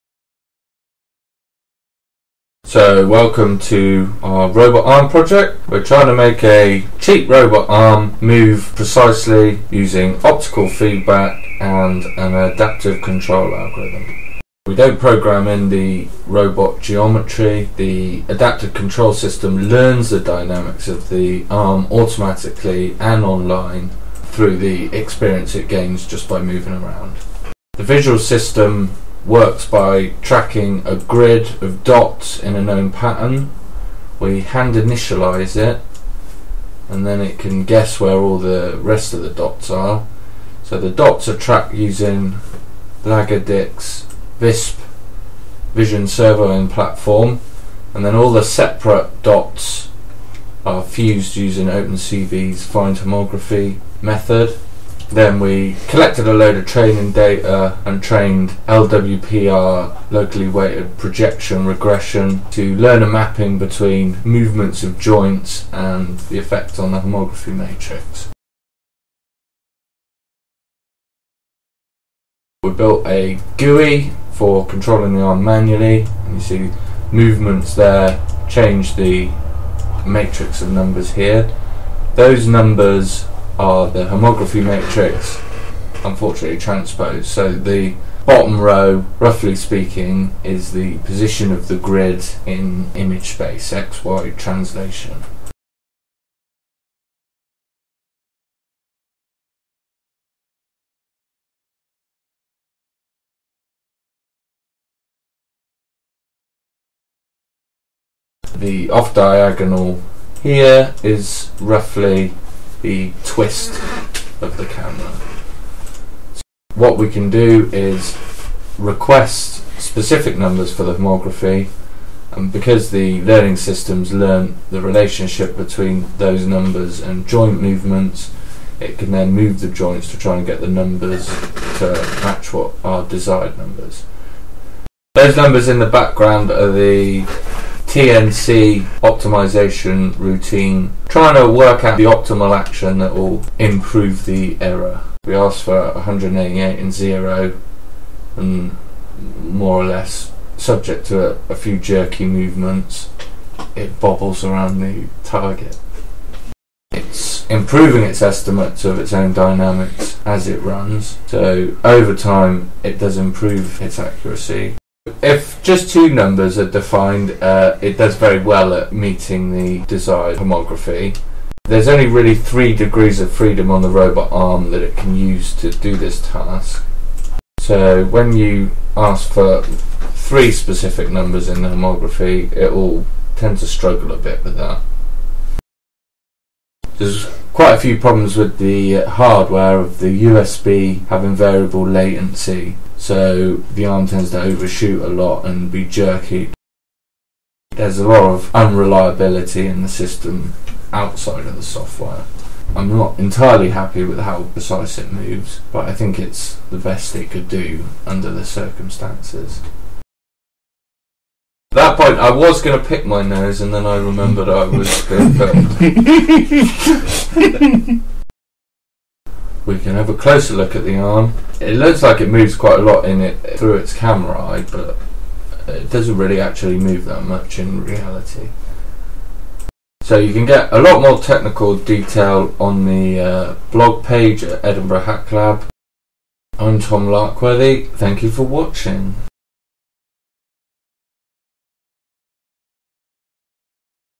Towards Precision on the cheap: webcam adaptive control robot arm
At Edinburgh Hacklab we are trying to get precision motion out of really cheap components. This setup cost £48. We used a cheap webcam to close the loop on a cheap robot arm, using VISP, OpenCV and LWPR software. We used an adaptive control system, so the geometry of the robot arm was never measured, the robot learnt how to control itself online.